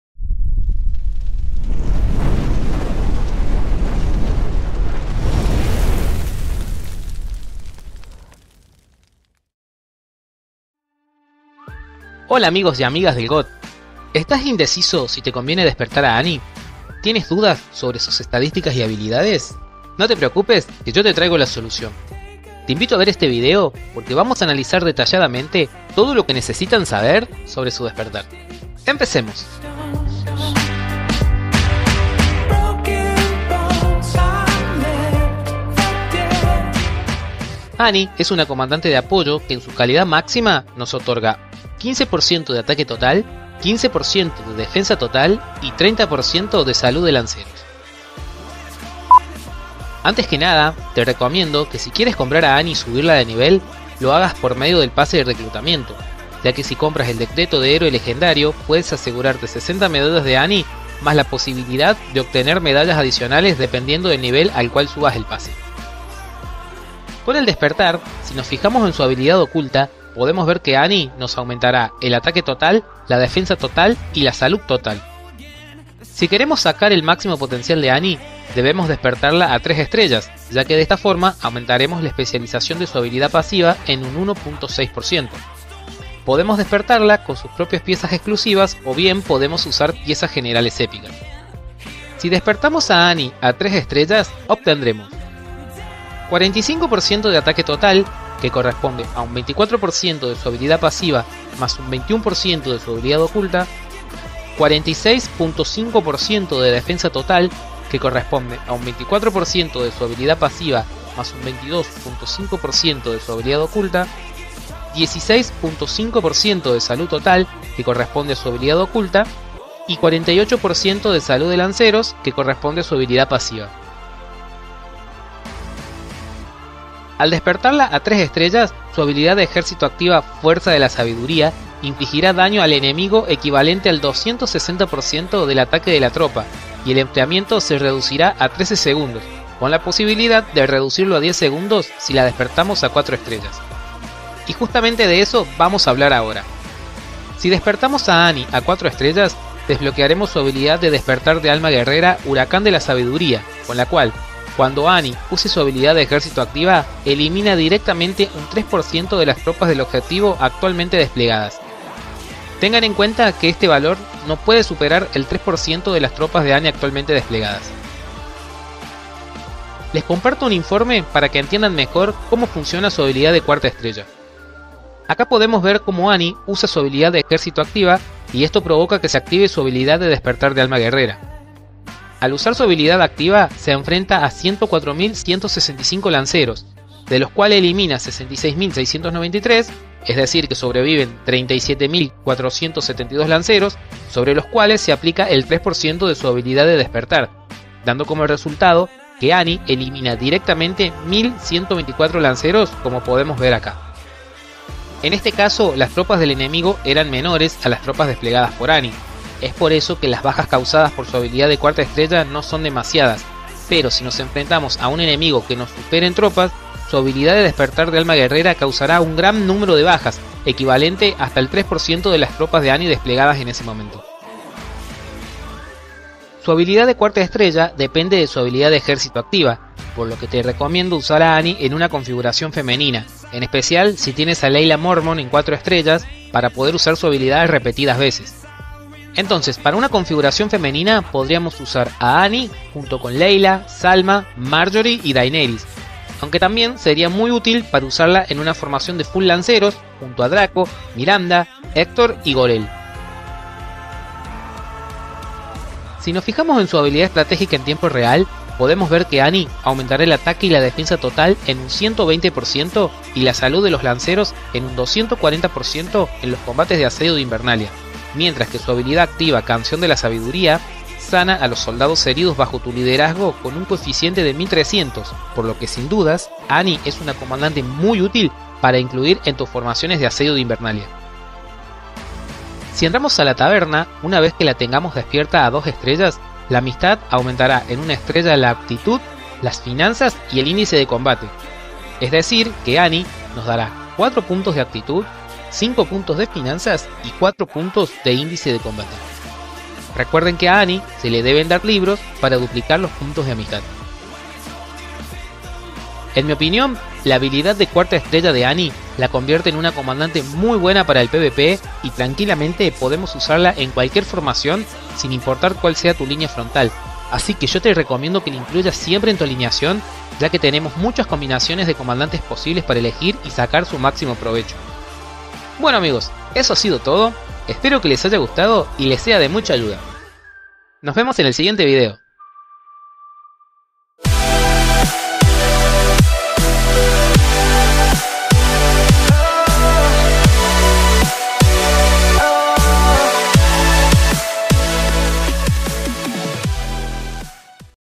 Game Of Thrones - GOTWIC - ANNIE AWAKENING - Review en español [ENG Sub]
Hola amigos! En este video les dejo un análisis de todo lo que necesitan
saber sobre el despertar de la Comandante Annie!
Hi friends! In this video I leave you an analysis of everything you need to
know about the awakening of Commander Annie!